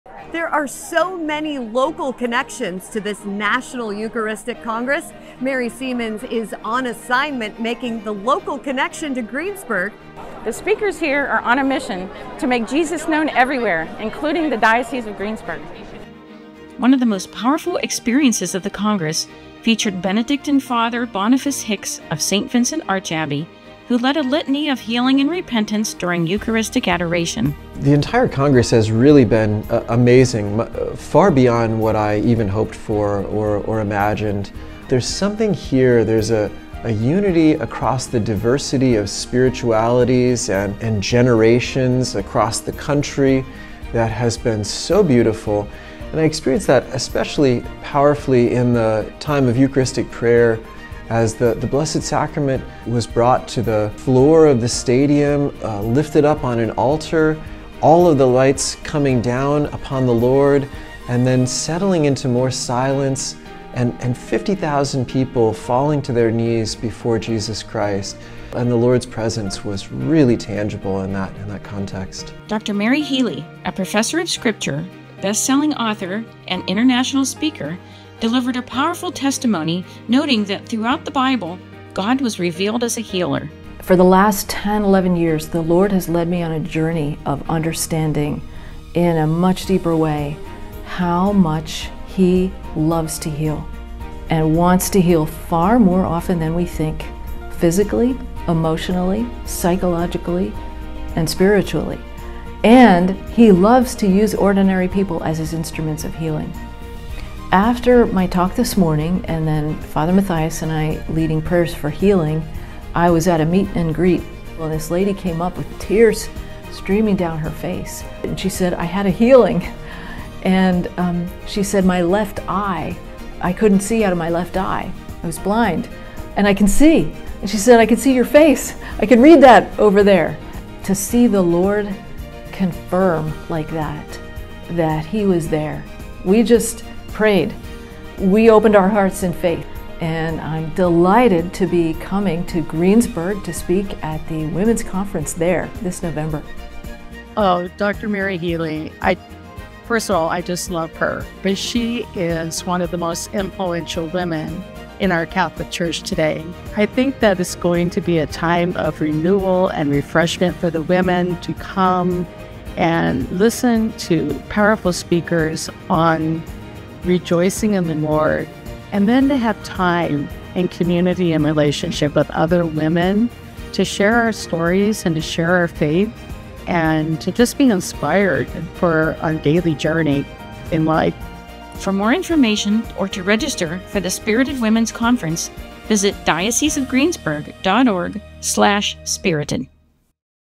The Local Connection to the National Eucharistic Congress
For the 60,000 Catholics attending the National Eucharistic Congress July 17-21 in Indianapolis, Ind., the message was clear: They were to leave Lucas Oil Stadium as missionary disciples, sent out to share the Good News of the Gospel with others Before they were sent out on mission, they heard from a variety of inspiring speakers whose goal was to make Jesus known everywhere, including the Diocese of Greensburg.
To read the full story visit TheAccentOnline.org